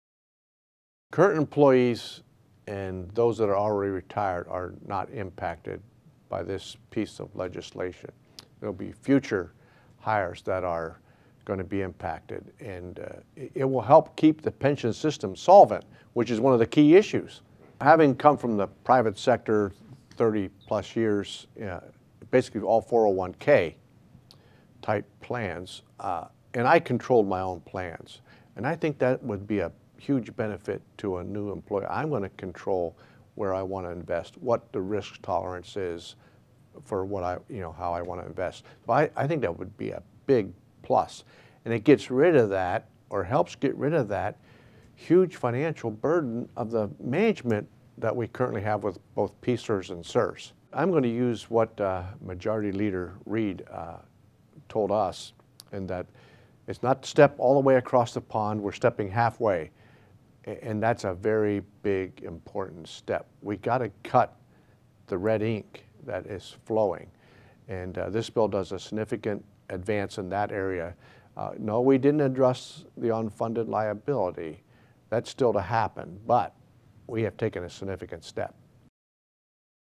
House Passes Historic Pension Reform
Pa. State Rep. Will Tallman makes comments about Pension Reform the House and Senate recently passed. Senate Bill 1 will balance the risk between taxpayers and employees.